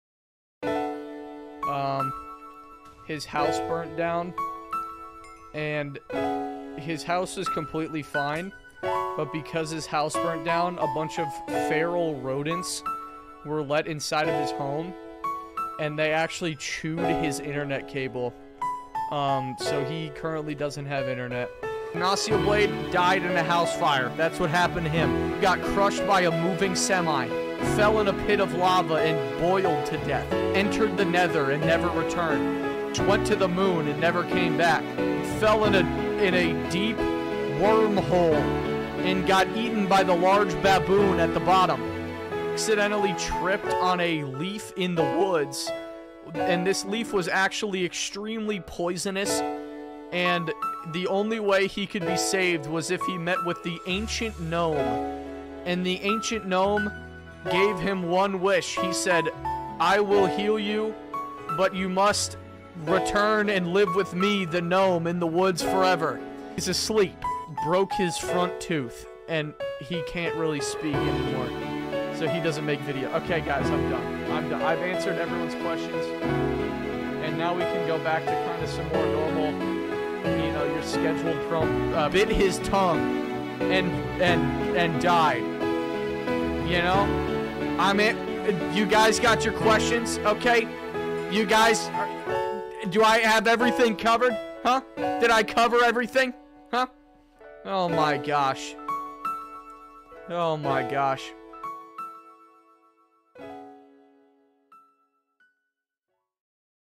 Evident Explains What Happened to Ignacio
Evident explains what happened to Ignacio from from Hive Skywars and treasurewars in Minecraft PVP games. Evident and Ignacioblade are Hive Minecraft Youtubers